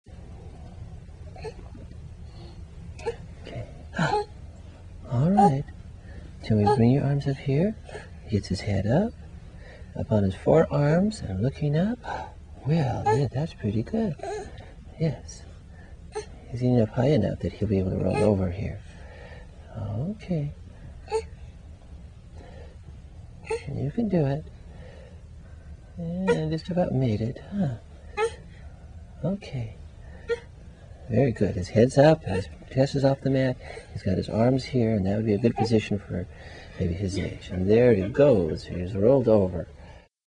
Neurology Exam, 3 Month: Positions - Prone
In the prone position, the baby is now able to bring his head up and look forward with the head being 45 to 90 degrees off the mat. Weight is borne on the forearms. When the head and chest are well off the mat, the baby is ready to start to roll from the prone to the supine position. Rolling front to back usually occurs at 3 to 5 months of age. Rolling over too early can be due to excessive extensor tone.